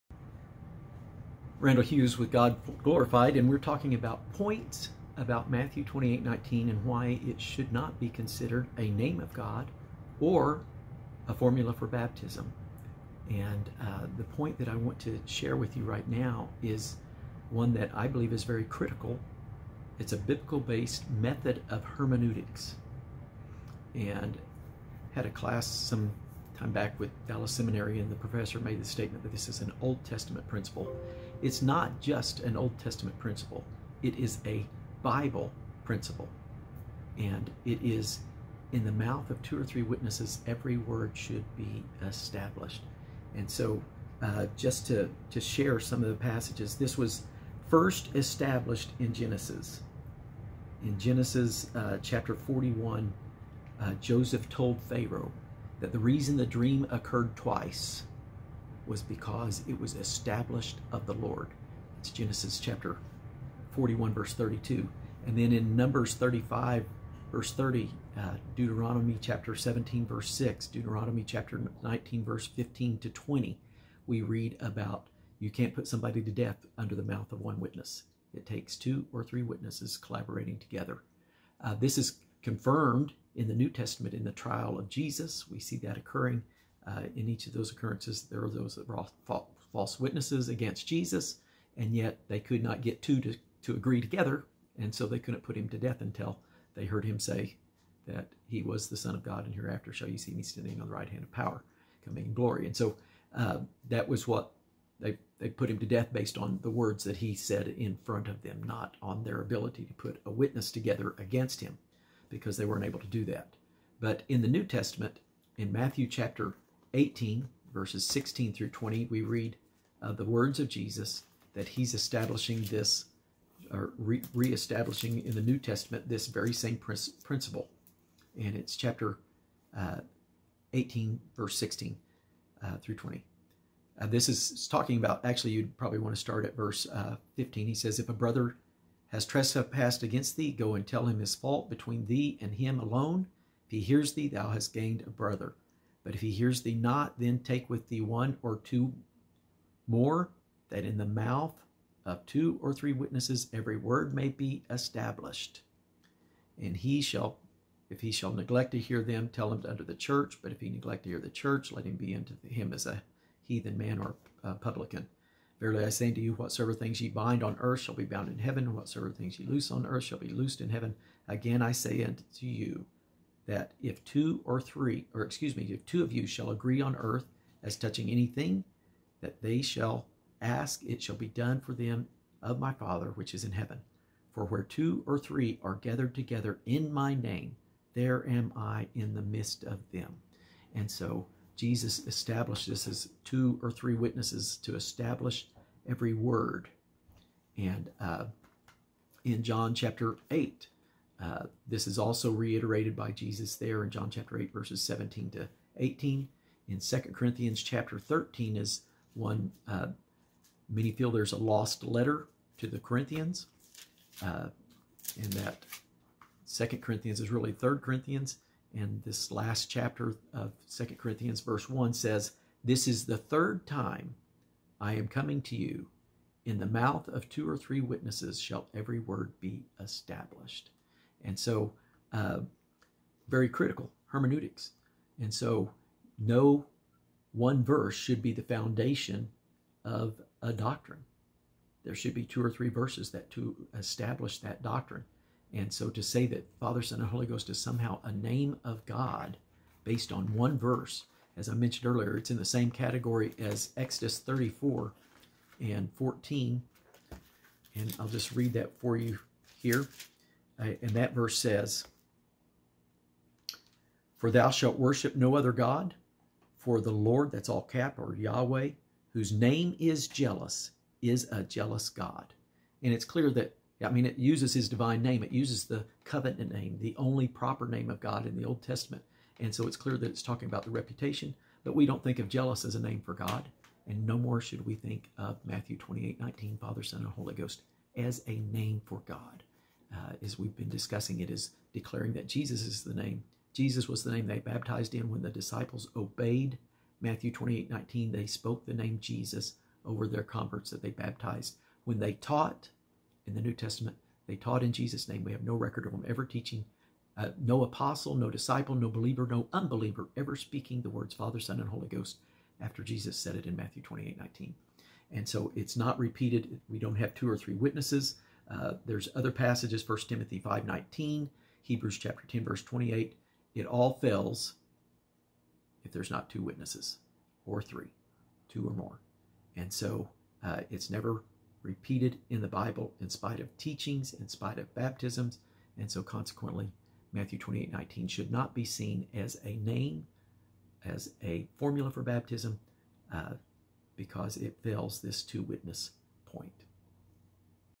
PT6 MT 28:19 and the Name for Baptism?
This is a discussion about the Name of God in the Bible centered around Matthew 28:19.

     "Go ye therefore and teach all nations, baptizing them in the name of the Father, and of the Son,    and of the Holy Ghost:"

Some points of discussion: Is Father, Son and Holy Ghost a name of God? Was this ever used for Baptism in the New Testament? Other verses compared to Matthew 28:19. Statistics of the Proper names referring to God. Is baptism a work? Jesus is the lamb of God that takes away the sins of the world. The Father did not die, and the Spirit did not die, so why would you be “buried with ‘them’ Romans 6:3?